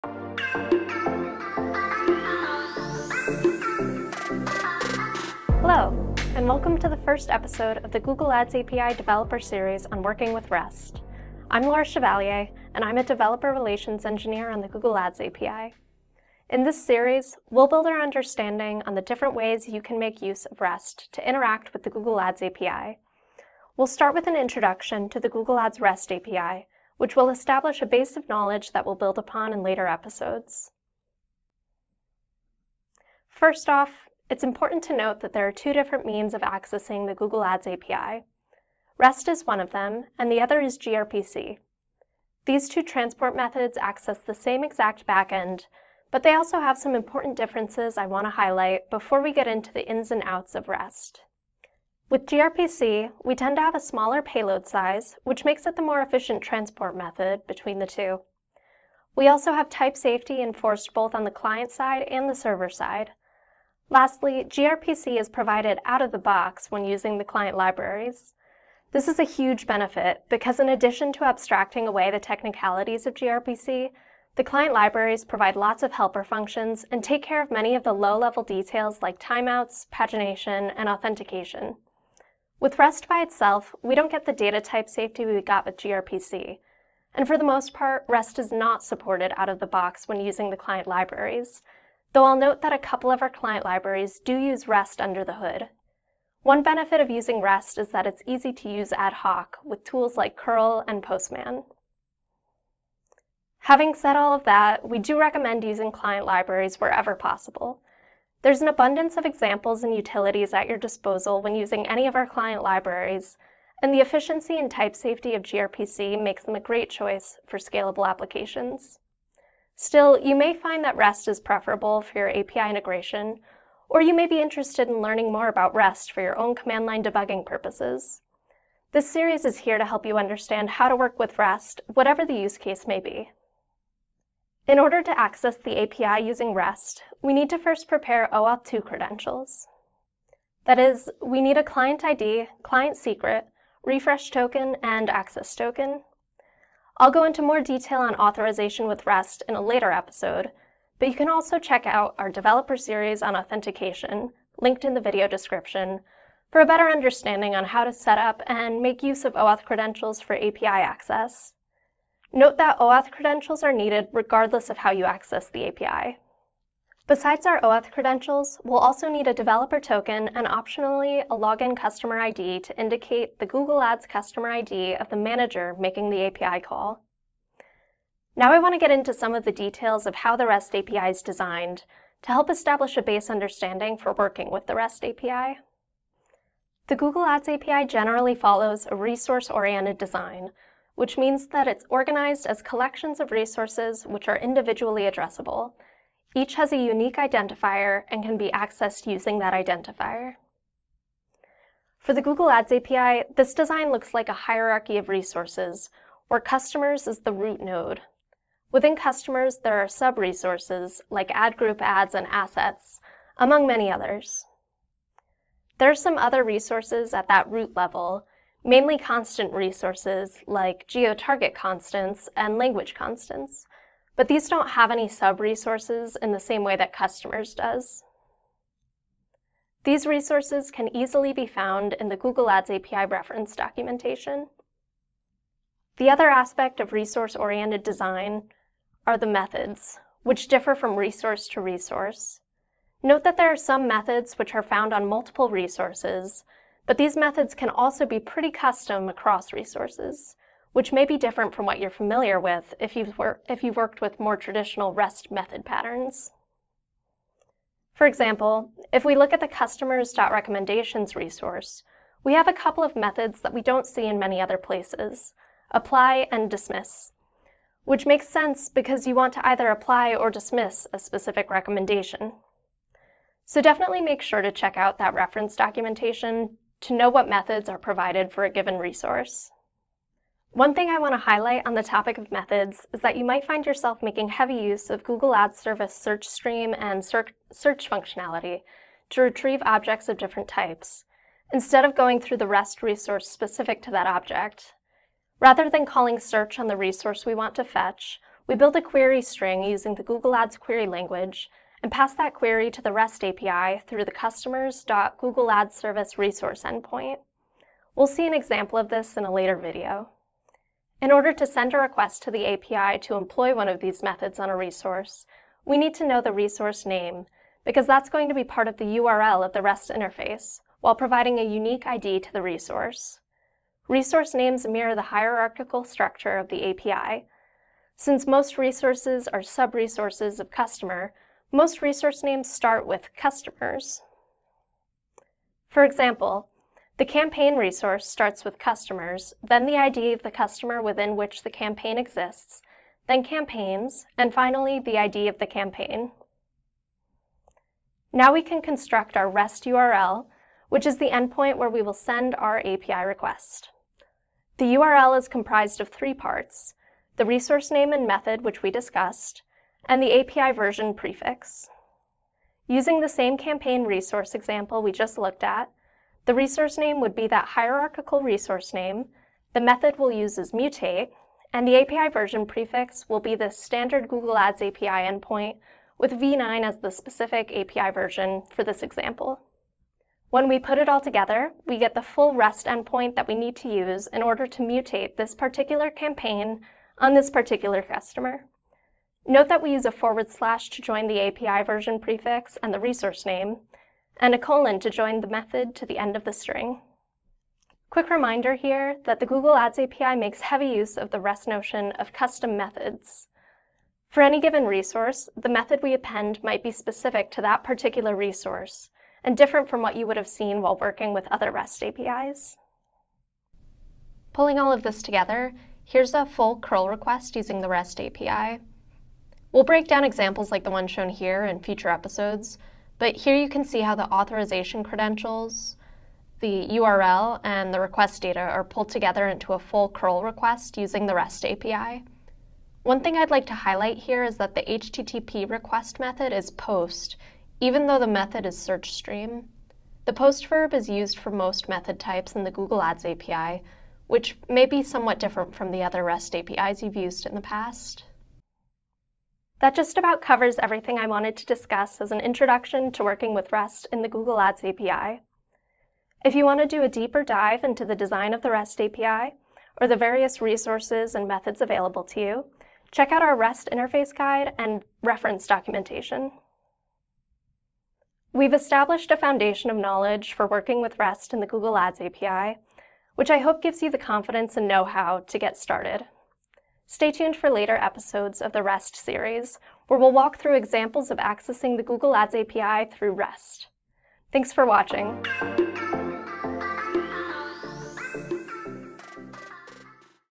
Introduction to Using the REST Interface with the Google Ads API
In this video, we’ll provide an introduction to using the REST interface of the Google Ads API. Topics include: transport choices of REST vs gRPC, necessary credentials, API design including resource naming hierarchy, and using cURL with the Google Ads API.